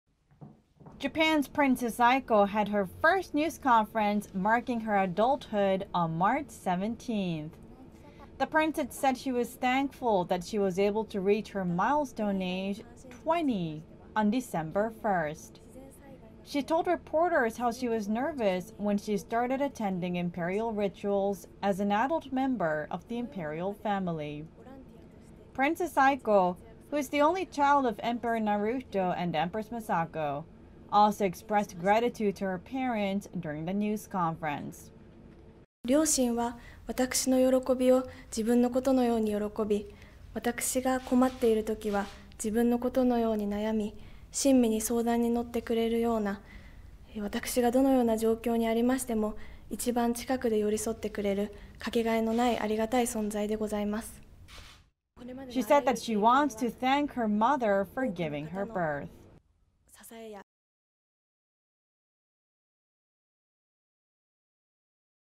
Japan's Princess Aiko has first news conference marking adulthood
Japan's Princess Aiko has her first news conference marking her adulthood.